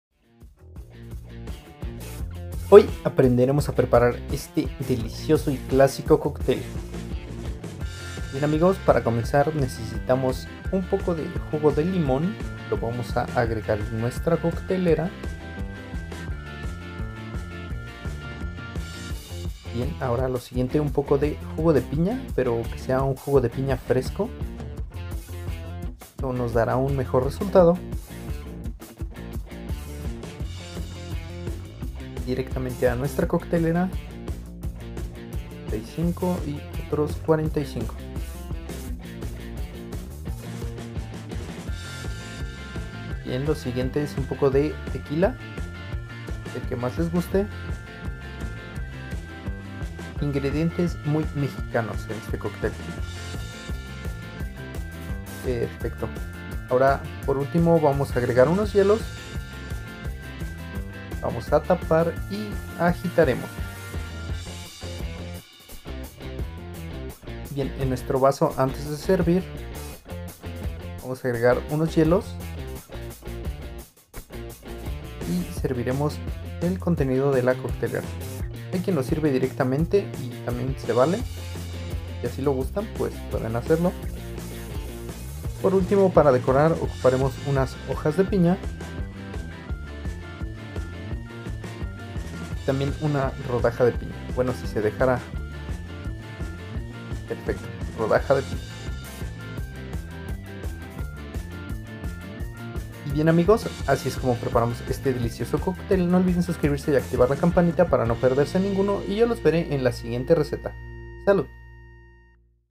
Matador - Cocteles con Tequila - Cocteleando
Matador
Hielos
15ml Jugó de Limón
90ml Jugó de Piña
50ml Tequila
Rodaja de Piña